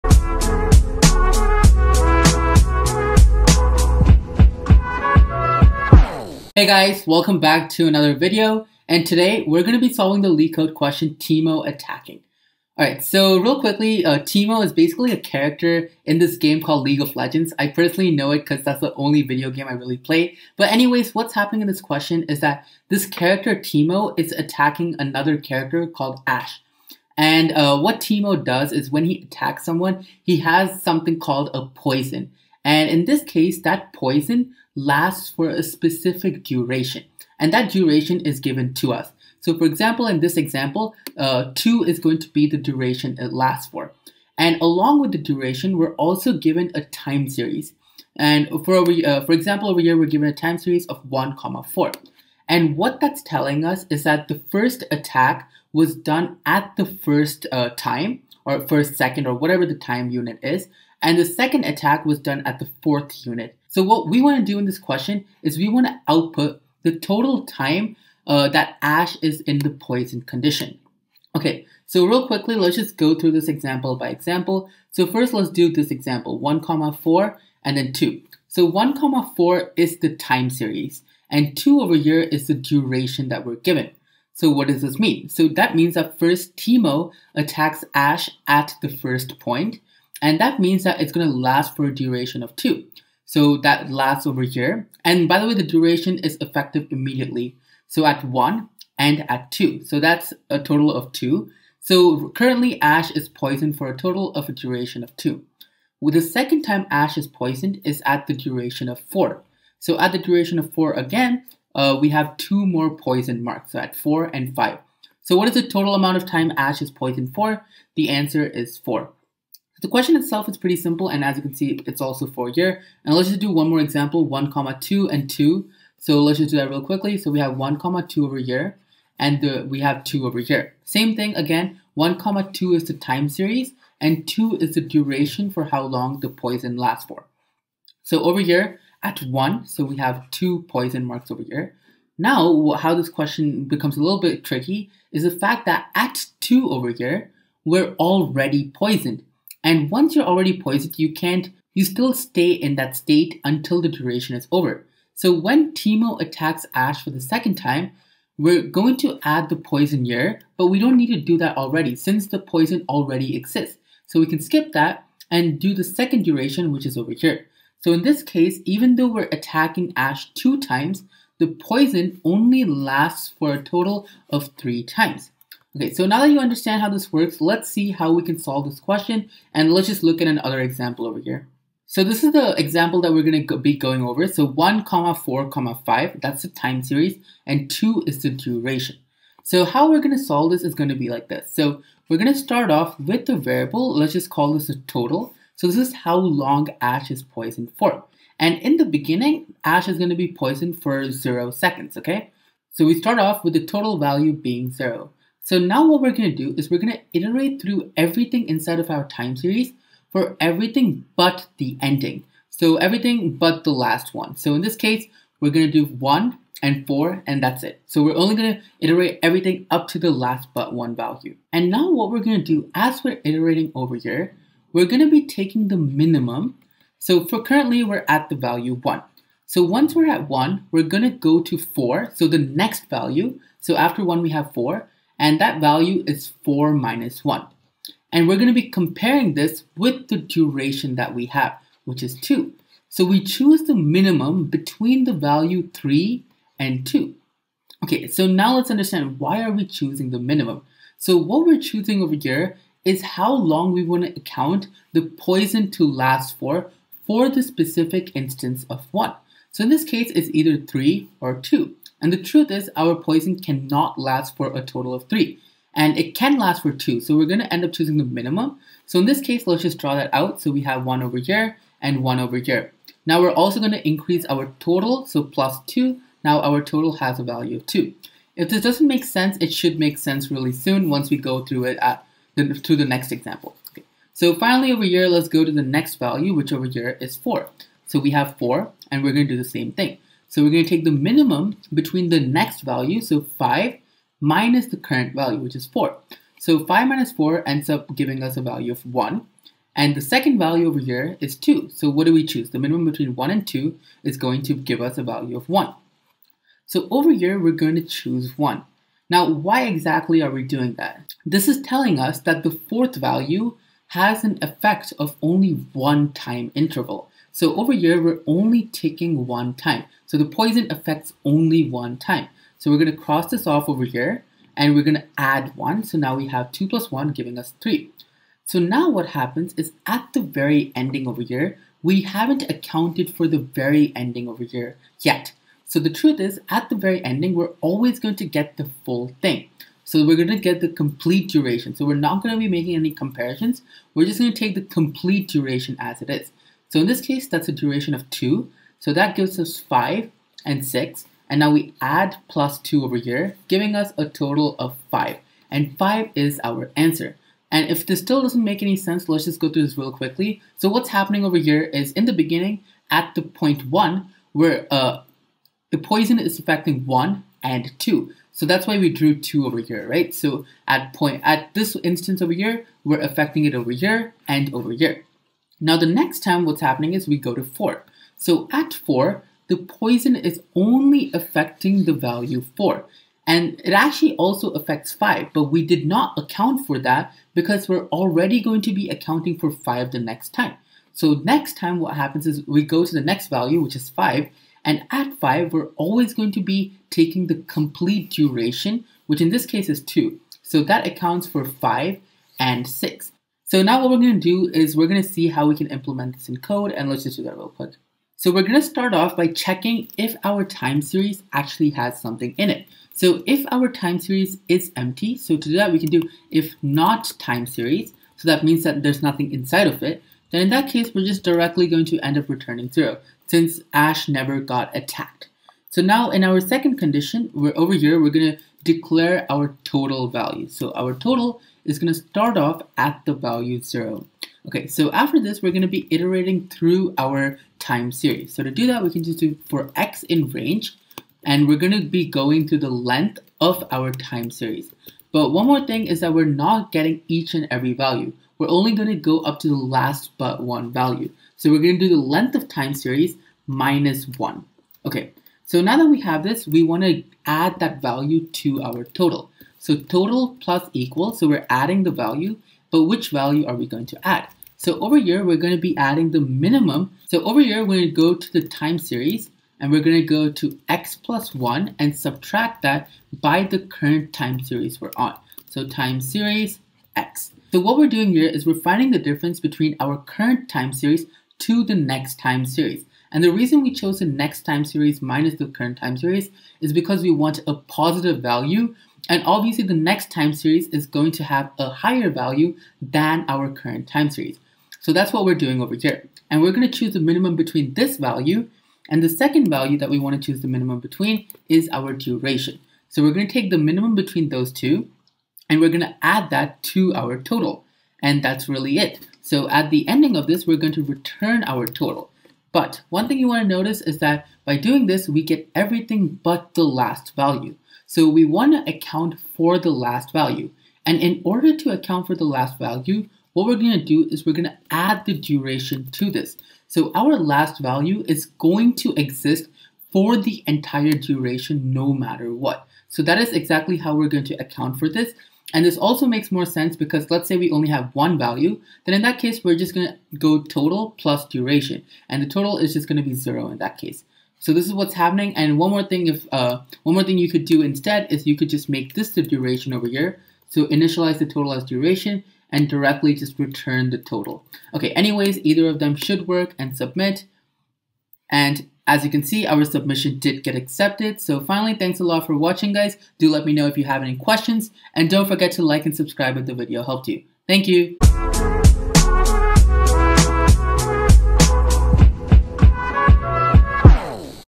Teemo Attacking | Leet code 495 | Theory explained + Python code
This video is a solution to Leet code 495, Teemo Attacking. I explain the question and the best way to solve it and then solve it using Python.

Timestamps:
0:00 Question Explained
2:57 Solution Explained
6:47 Python Code

Gear used:
Drawing pad